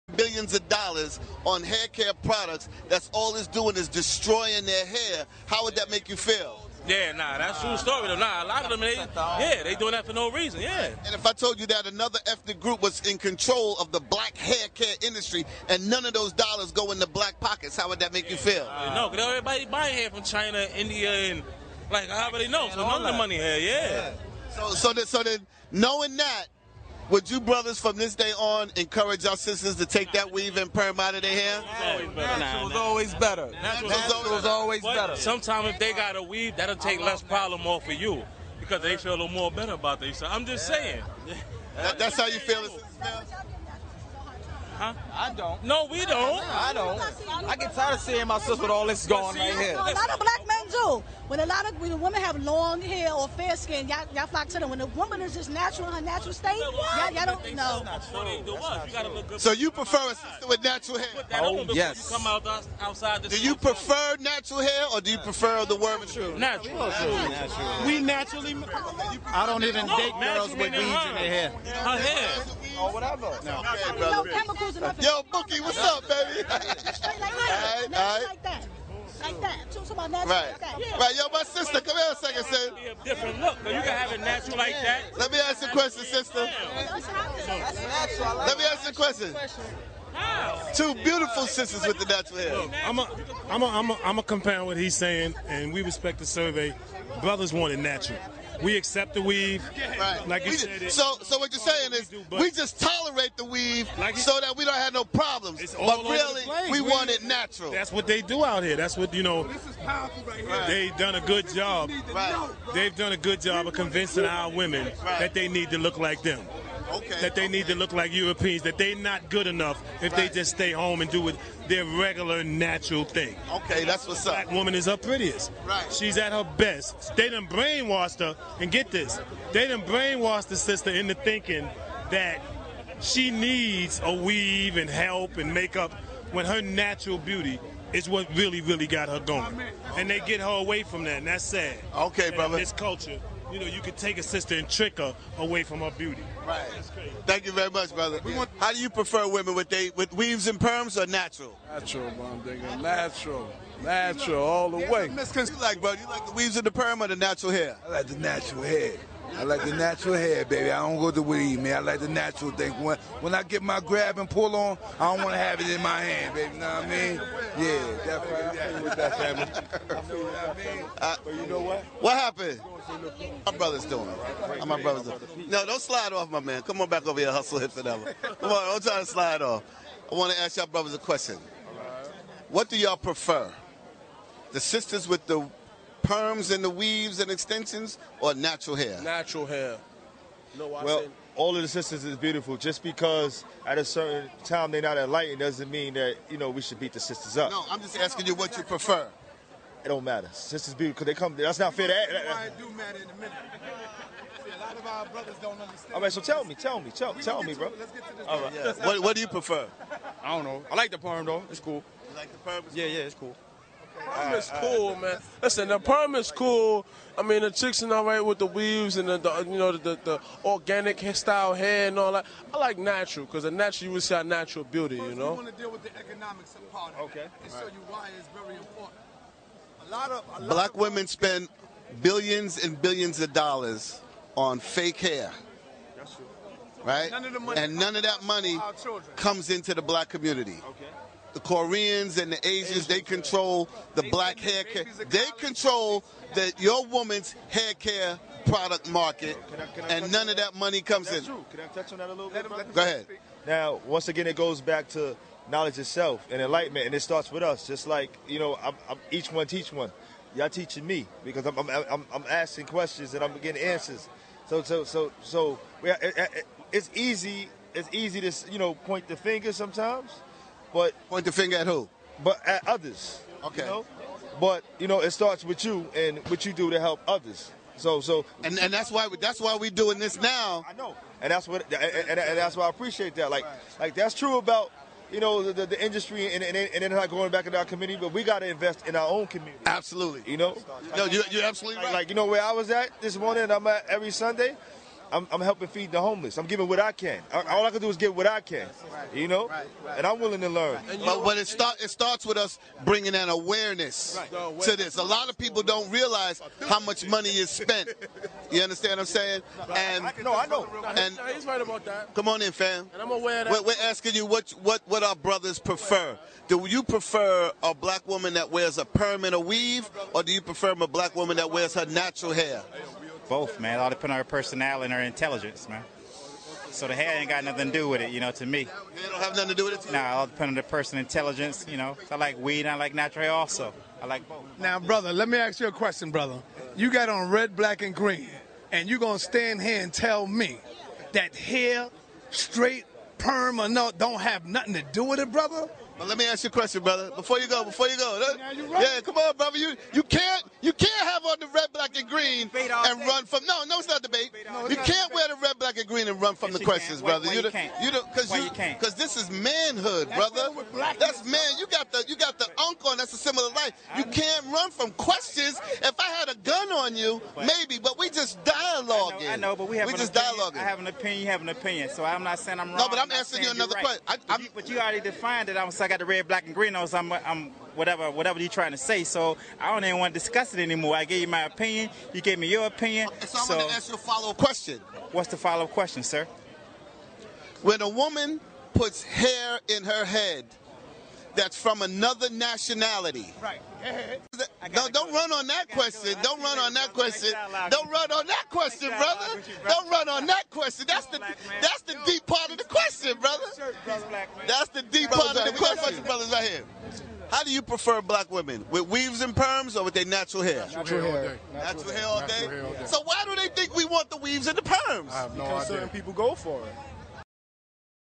Sistas Are Going Natural... (part 4 of 4)
The self identity crisis is real, natural hair has never caused our women cancer nor has it made them broke; sistas, we wouldn't tell you, if we didn't love you, for the video